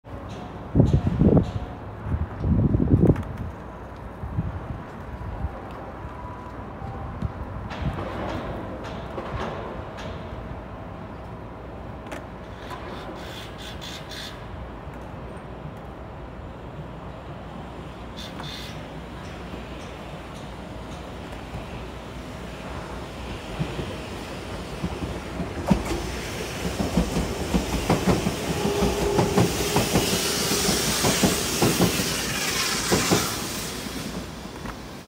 (freight) 2022/02/05 試****M E491 East i-E
(Operation No, train info)
- E491 East i-E

On the Takashima line.

(MUDO is Non-powered train)
(KOKI is a container freight car.)
(TAKI is a  tanker freight car.)
(Yo is a caboose.)